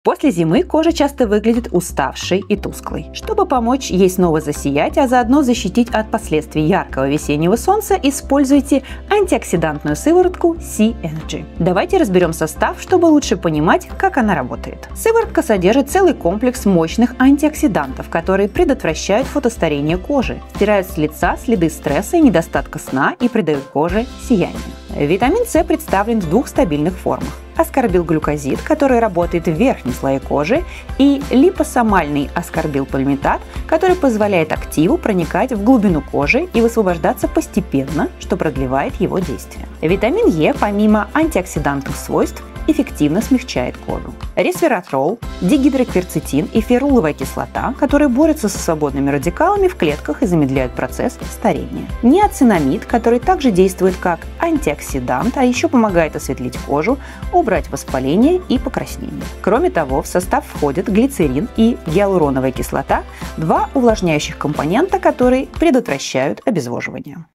Сияние кожи с антиоксидантной сывороткой С-Energy
Для того, чтобы помочь коже адаптироваться к погодным изменениям и солнцу, замедлить фотостарение, выровнять тон кожи и придать ей сияние, добавьте в уход антиоксидантную сыворотку С-Energy.